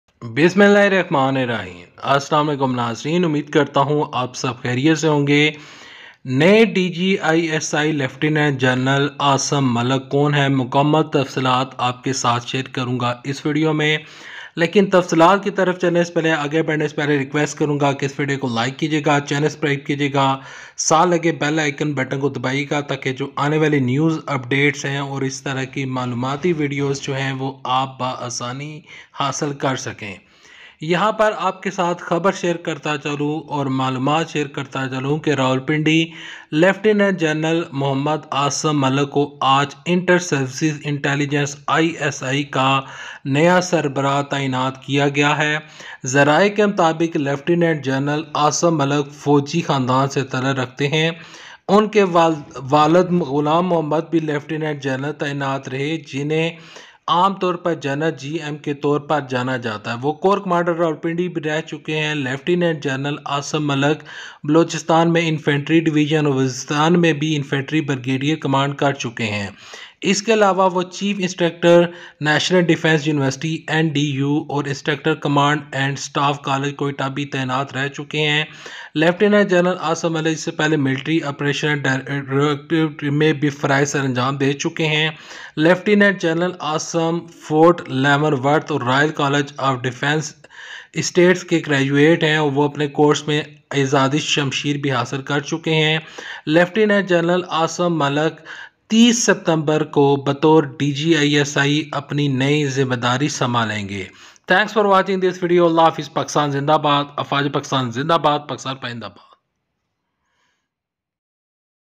Who is Newly Appointed DG ISI Lieutenant General Muhammad Asim Malik ? biography & Complete details!
نئے ڈی جی آئی ایس آئی لیفٹیننٹ جنرل عاصم ملک کون ہیں؟
جانیے مکمل تفصیلات ا

LieutenantGeneralMuhammad Asim Malik has been appointed as the Inter-Services Intelligence director general and will take charge on September 30

Gen Malik is currently serving as an adjutant general at the General Headquarters in Rawalpindi, the statement said.

He will be replacing LtGenNadeemAnjum, who was appointed in 2021 by then-prime minister #ImranKhan ! dgispr pakarmy ! Details by Amad majeed!